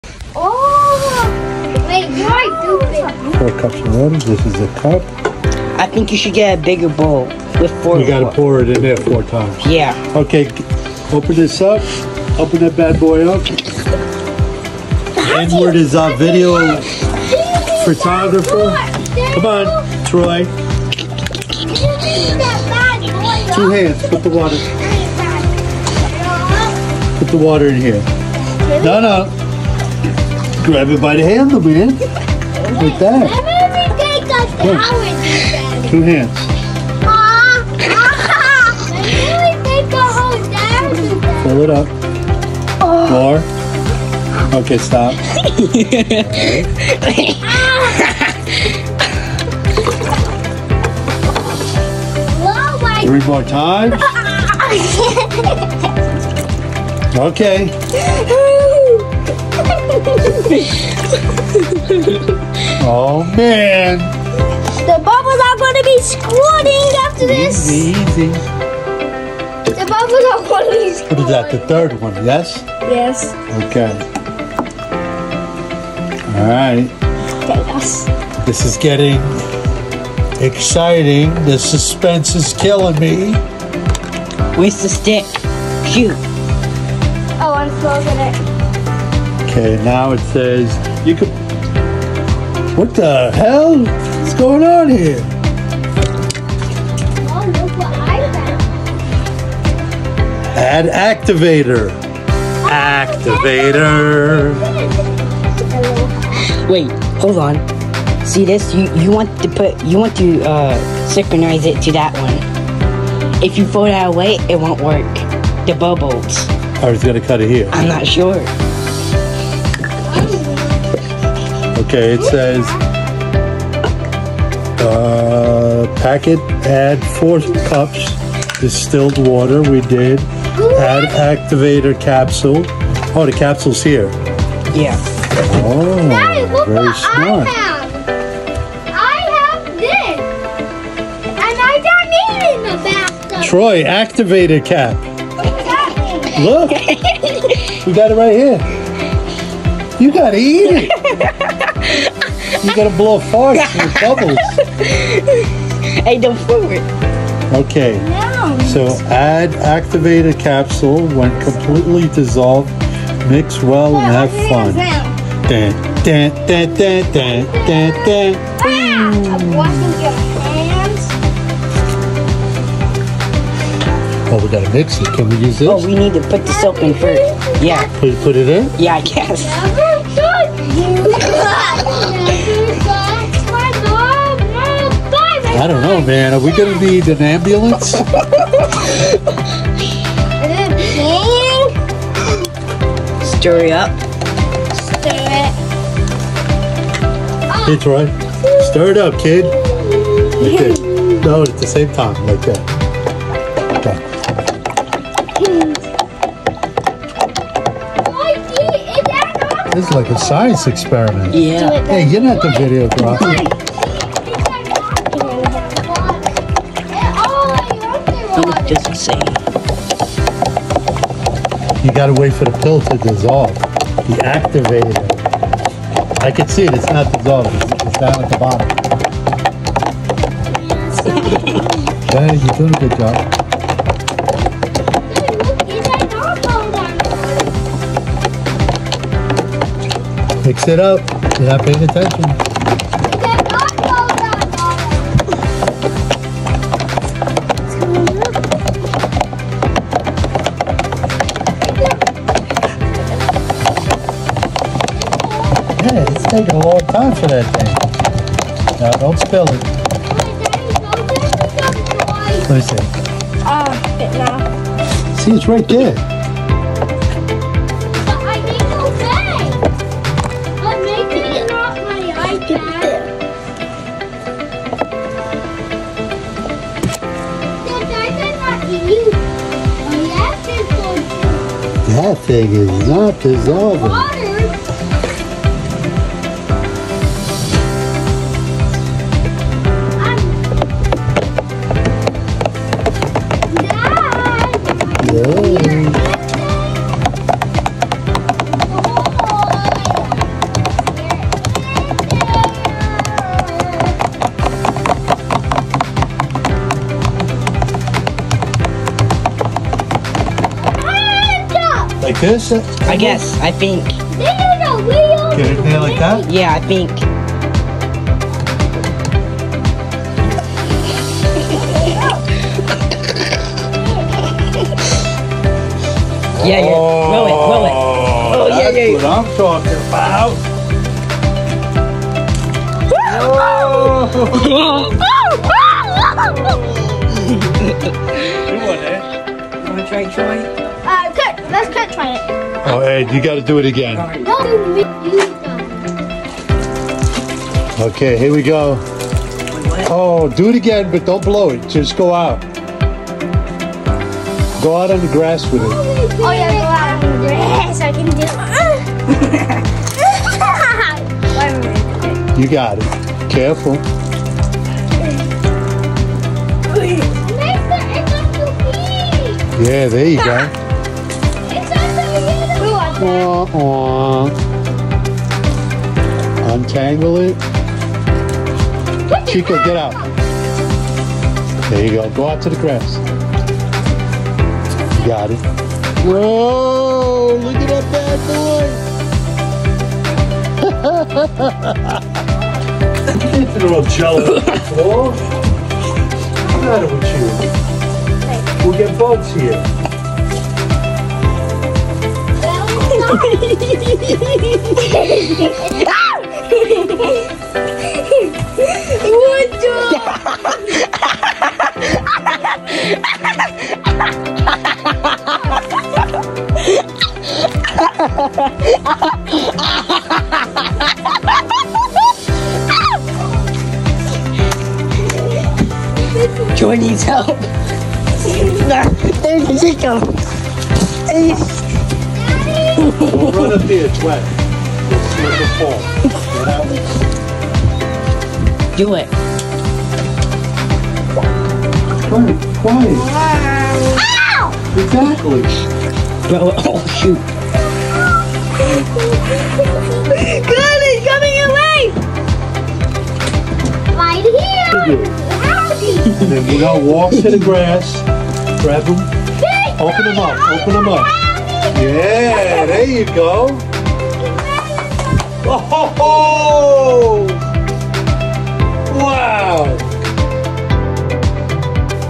Testing out the Aloha Bubbles🫧🫧 Hawaii!!
Aloha bubbles is a form of liquid that makes bubbles into big ones￼!!!